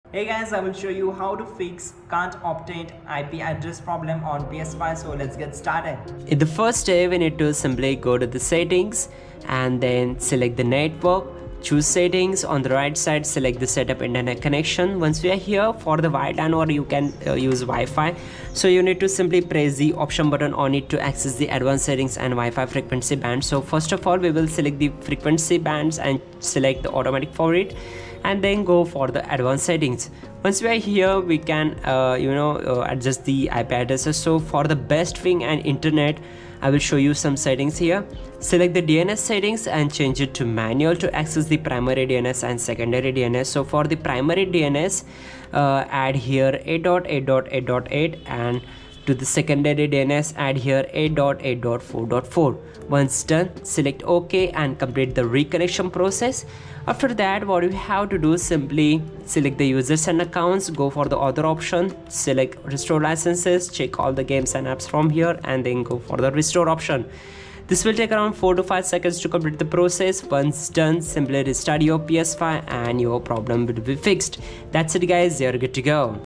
How to Fix Can't Obtain IP Address on PS5 ⚡playstation5
How to Fix Can't Obtain IP Address on PS5
If your PS5 can't obtain an IP address, it may be due to network issues, incorrect settings, or router problems. To fix this, start by restarting both your router and PS5, then try reconnecting. How to Fix Can't Obtain IP Address on PS5 - If you’re using Wi-Fi, move your PS5 closer to the router or switch to a wired connection for better stability. You can also manually set up your connection by going to Settings then Network then Set Up Internet Connection then Custom, selecting Manual, and entering an IP address within your router’s range along with DNS settings like 8.8.8.8 (Primary) and 8.8.4.4 (Secondary). If you're using an Ethernet cable, try a different LAN port on your router or swap out the cable. Additionally, check your router’s MAC address filtering settings and disable them if necessary. If the issue persists, reset your PS5’s network settings in Settings then Network then Reset Network Settings and ensure your internet service is active. If nothing works, contacting your ISP or PlayStation Support may be necessary for further troubleshooting. How to Fix Can't Obtain IP Address on PS5